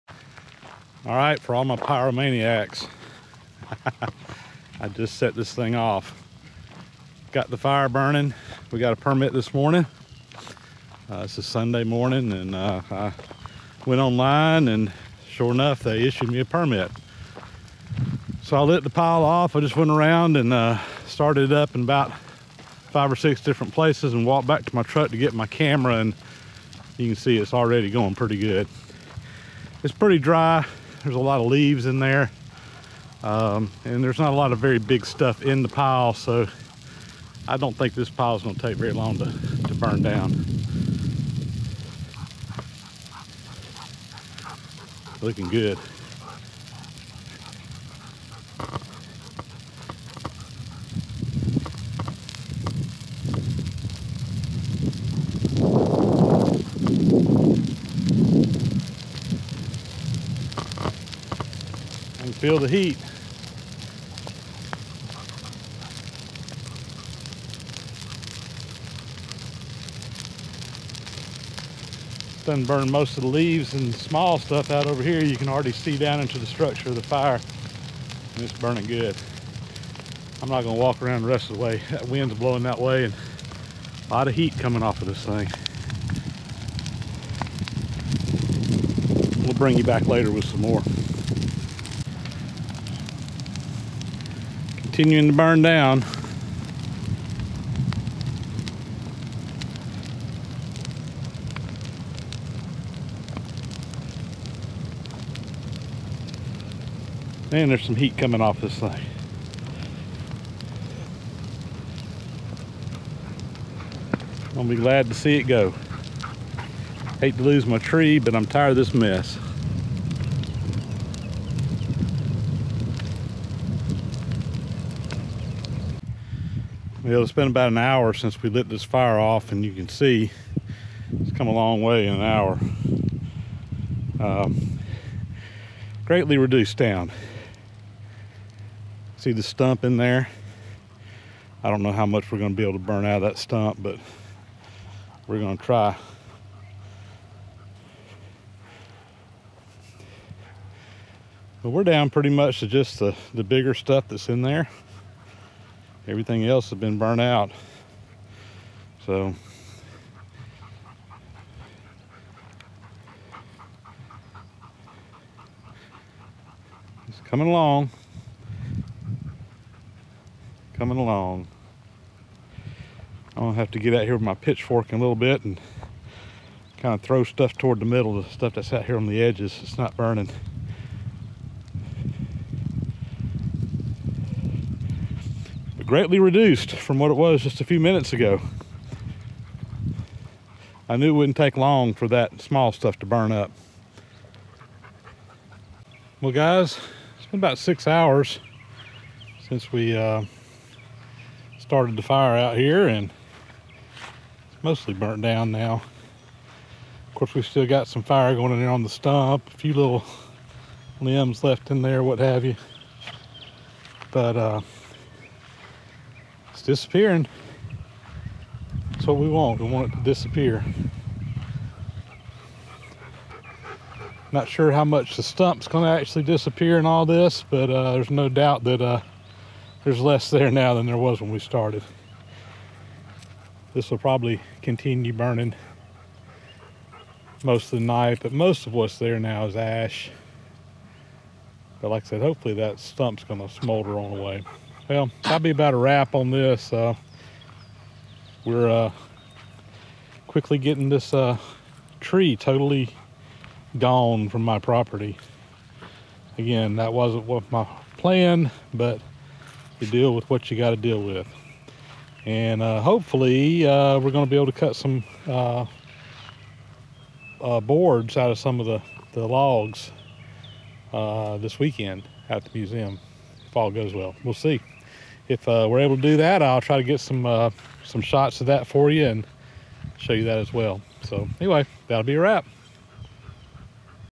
Storm Damage 4: Burn Pile
Some footage of the burn pile going up for all of my pyromaniac fans!